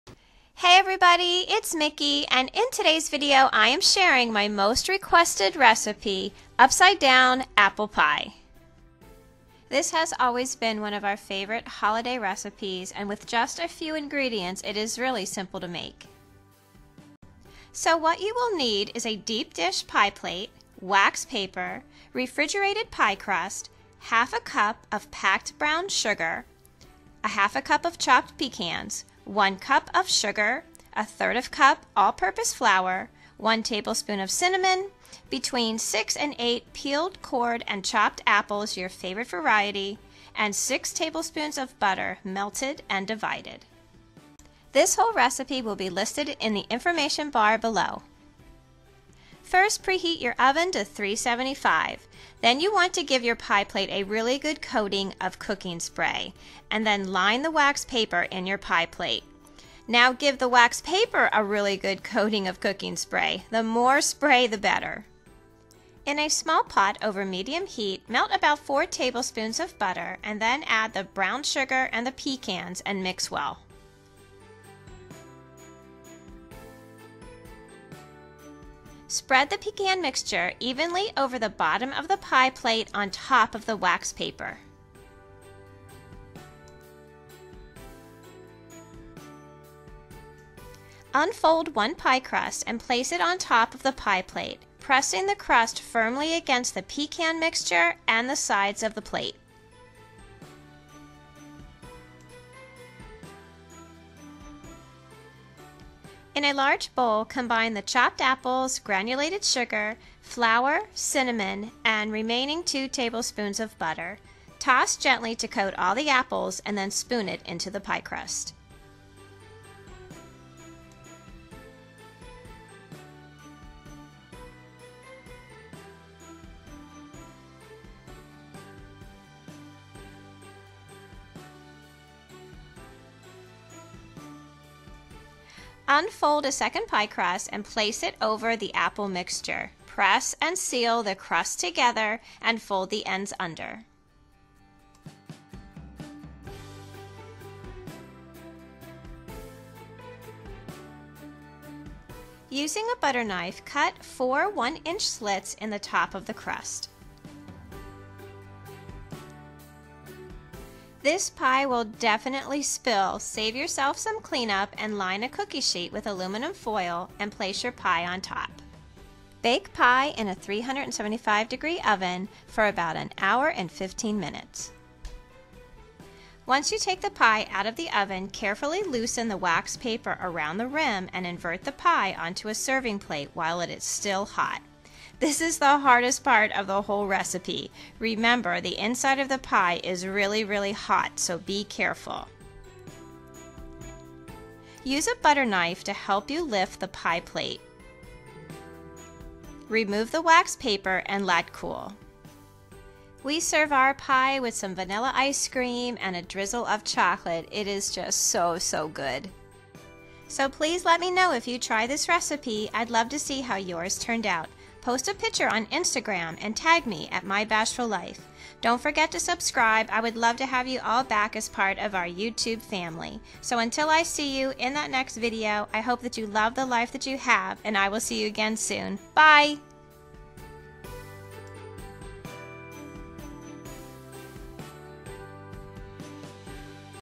Upside Down Apple Pie / Holiday Baking
In Today's Holiday Baking video  I am sharing my most requested recipe, Upside Down Apple Pie.  This recipe is super easy with only a few ingredients and is so yummy, you may never make right side up pie again!  Recipe Below.

Upside Down Apple Pie
6 tblsp butter melted divided
1/2 cup packed brown sugar
Refrigerated pie crust
1 cup sugar
1/3 cup flour
1 tsp cinnamon
6-8 peeled cored chopped apples
Preheat oven to 375.  Coat a deep dish pie plate with nonstick cooking spray and line it with waxed paper.  Coat the waxed paper with cooking spray.  The more spray the better
In a small pot over med heat combine 4 tables butter, brown sugar and pecans, mix well.  Unfold 1 pie crust and place  it in the pie plate, pressing against the pecan mixture and up the sides of the plate.
In a large bowl combine chopped apples, granulated sugar flour cinnamon and remaining 2 tabls butter mix well.  Spoon into prepared pie crust.  Unfold second pie crust and place over the apple mixture.  Press edges to seal and turn under.  Using a knife cut 4 one inch slits in top of crust.
Bake in a 375 oven for about 1hour and 15 mins or until golden brown.  Carefully loosen the waxed paper around rim and invert pie onto a serving dish while still hot.  Remove the wax paper and allow to cool!

Camera I Use:
Canon Rebel SL1

I ALWAYS WRITE BACK;
My Bashful Life
Mickey